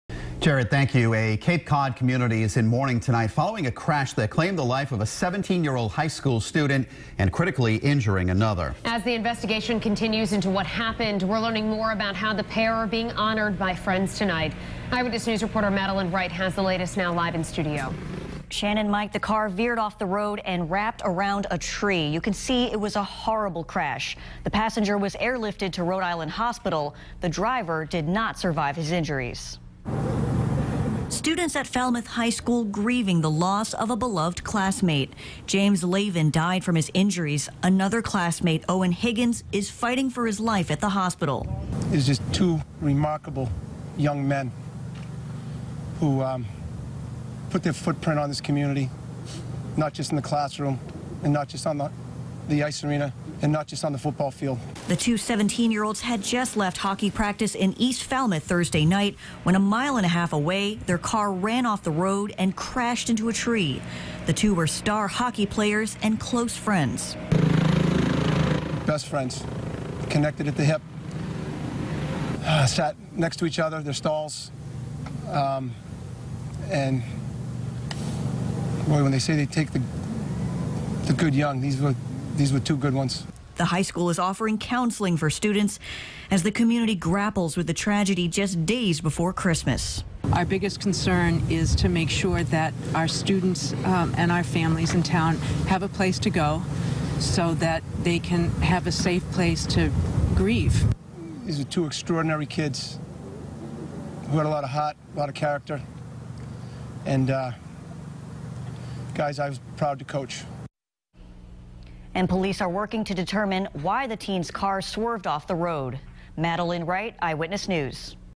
Police: High school student killed, another injured in crash
Police say the vehicle went off the road and hit a tree in Falmouth just before 7 p.m. Thursday.